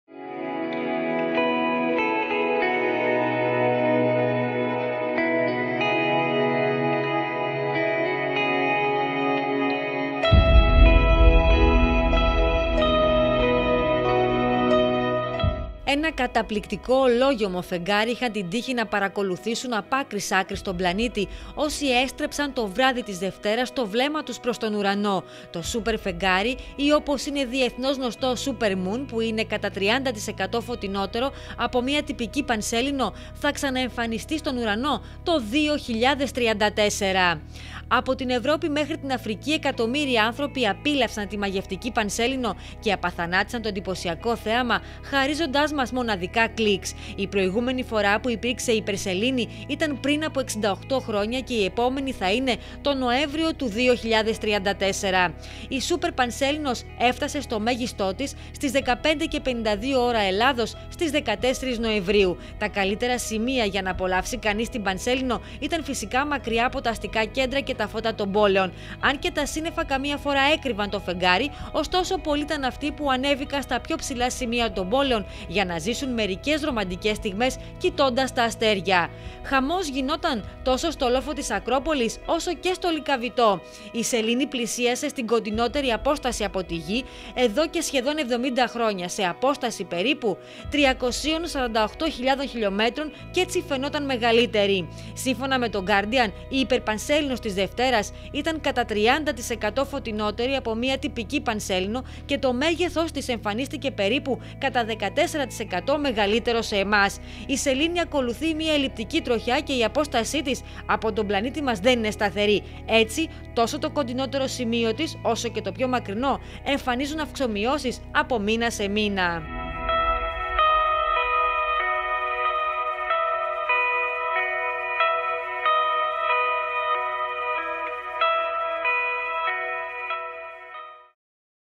Η ΜΕΓΑΛΥΤΕΡΗ ΠΑΝΣΕΛΗΝΟΣ ΤΟΥ 2016
Τη μεγαλύτερη πανσέληνο από το 1948 είχαν την ευκαιρία να απολαύσουν το βράδυ της Δευτέρας οι περισσότεροι άνθρωποι στον πλανήτη, από την Ευρώπη και την Αφρική έως την Αμερική και την Άπω Ανατολή.